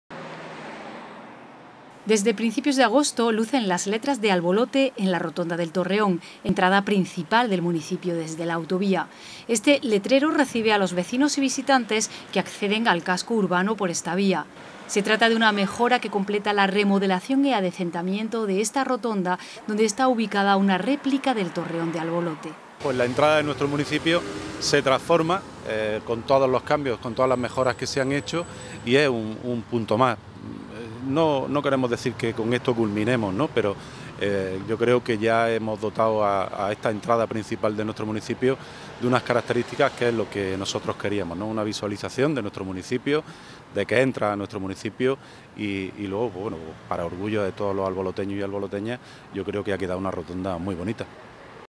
NUEVO LETRERO DE ENTRADA A ALBOLOTE EN LA ROTONDA DE EL TORREÓN
La palabra ALBOLOTE recibe a los que accedan por la entrada de la rotonda de El Torreón